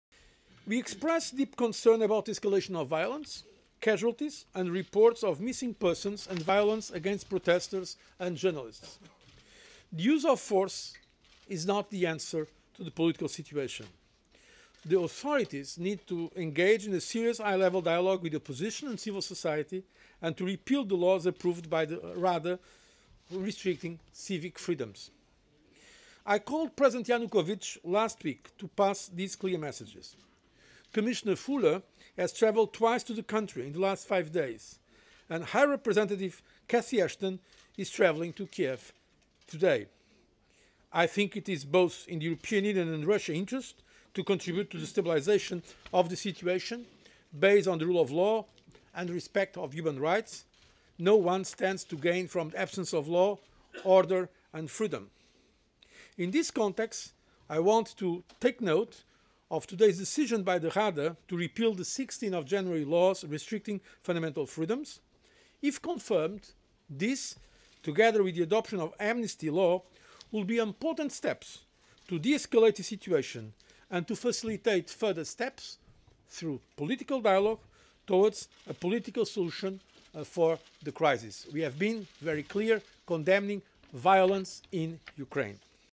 EU's Barroso welcomes abolishment of anti-protest law in Ukraine
Speaking at a press conference in Brussels after the EU-Russia summit on Tuesday, European Commission President Jose Manuel Barroso welcomed the decision of the Ukrainian parliament to abolish an 'anti-protest law' approved last week.